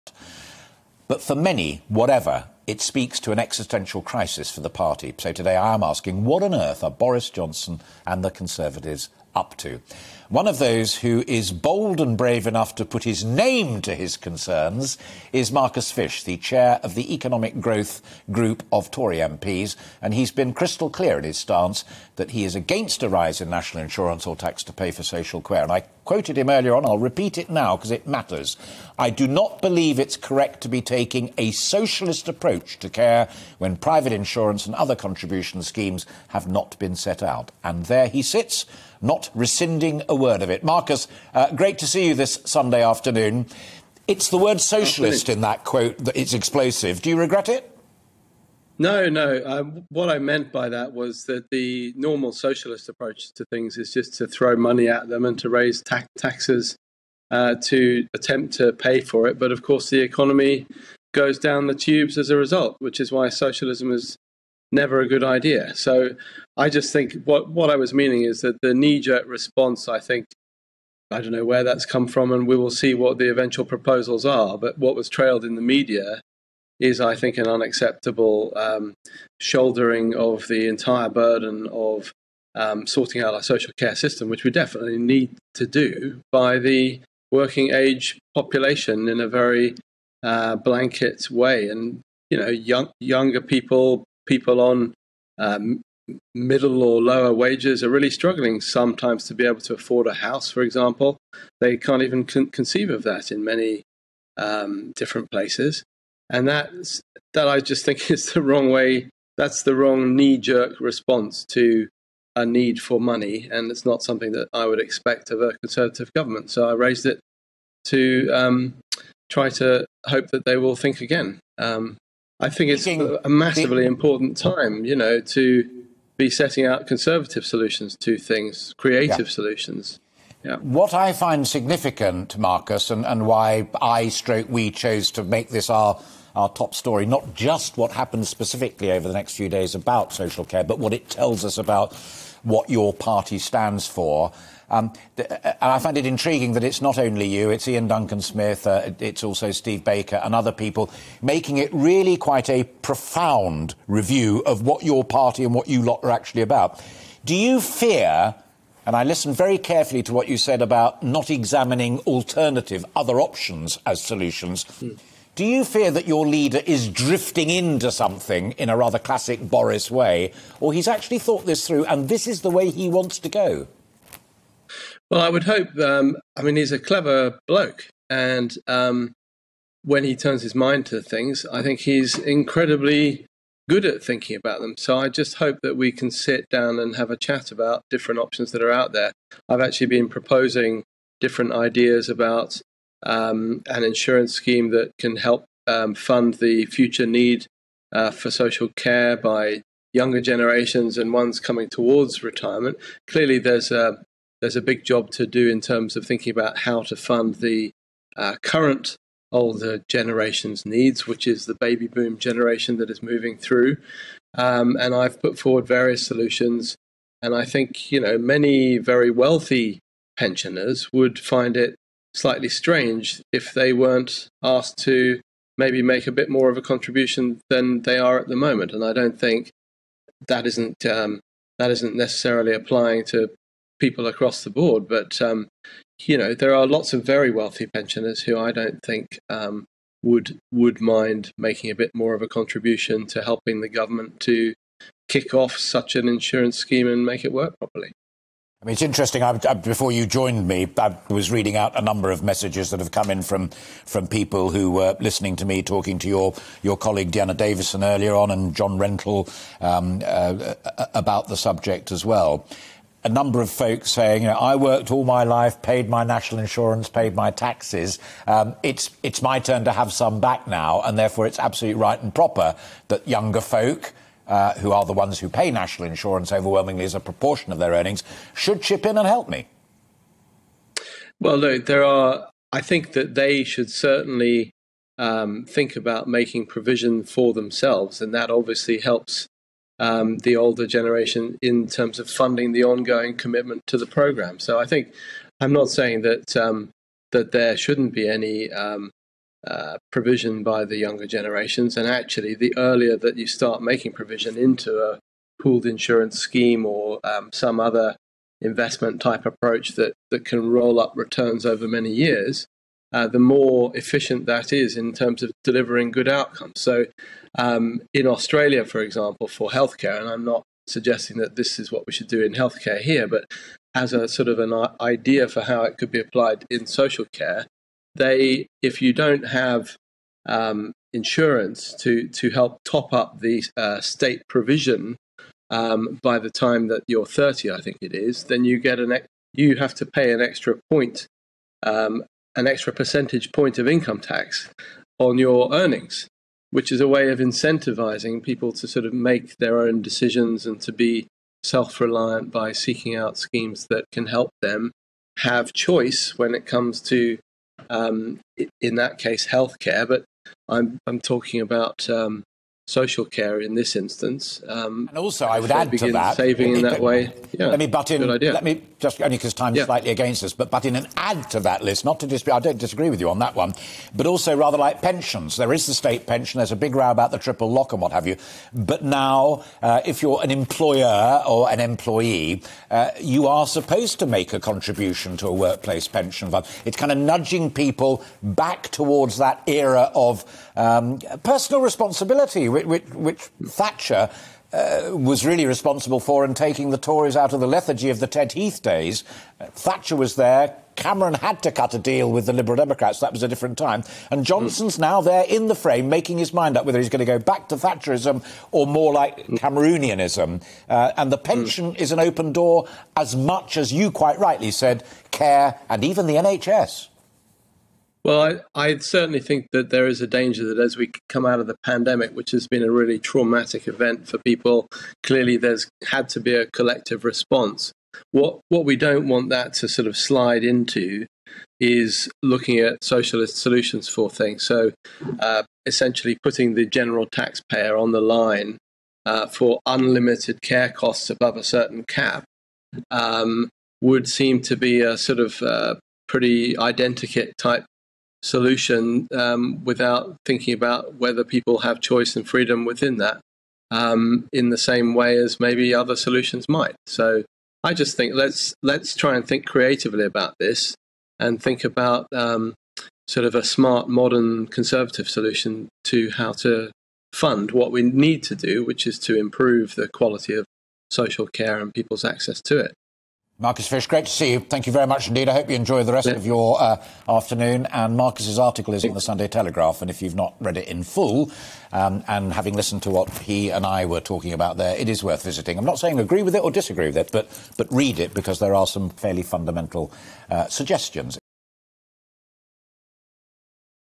Marcus Fysh: Normal socialist approach to problems is to throw money at them
'What was trailed in the media is, I think, an unacceptable shouldering of the entire burden of sorting out our social care system.'

Tory MP Marcus Fysh says potential government plans to address social care are not something he would expect of a conservative government.